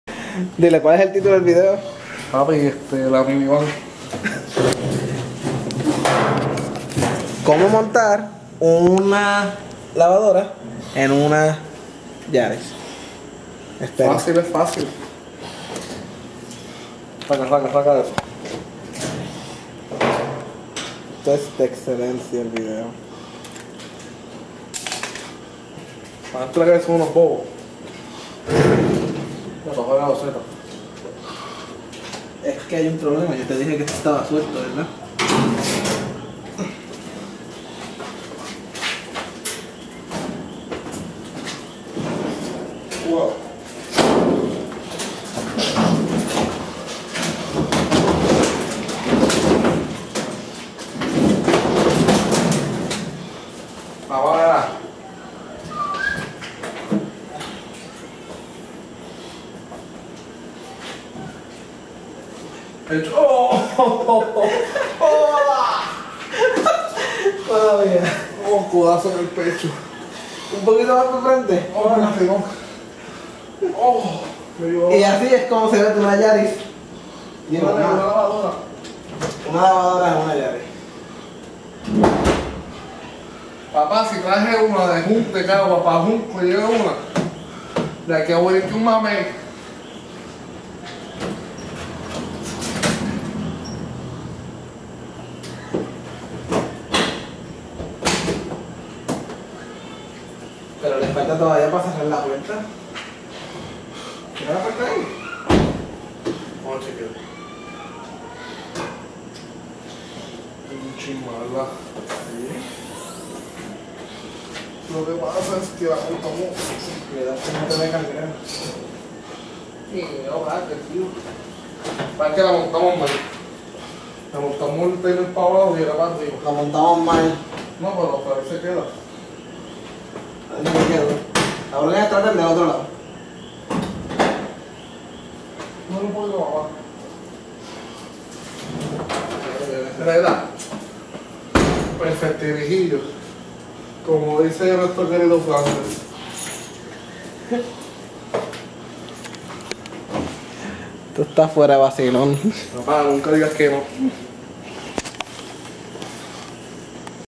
Como Montar Una Lavadora en una Yaris
Este video es como se monta una lavadora en una toyota Yaris ;) espero que lo disfruten el video esta crudo dado a que no tenia ganas de editar hoy.